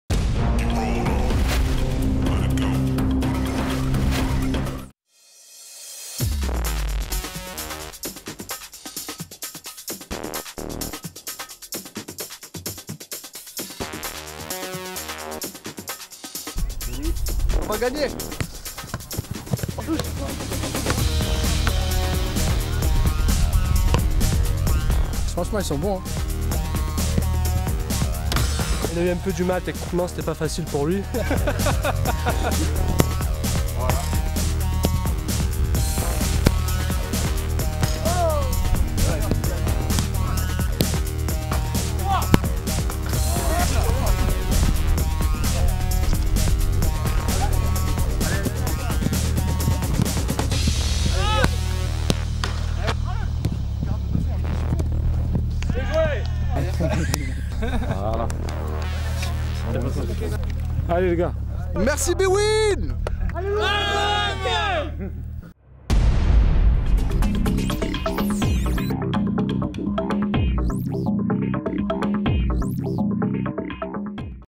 Dans la peau de l'OM (Olympique de Marseille)
Qualifiés via diverses promotions sur bwin.fr, ils ont pu rencontrer et jouer avec leurs idoles de l'Olympique de Marseille !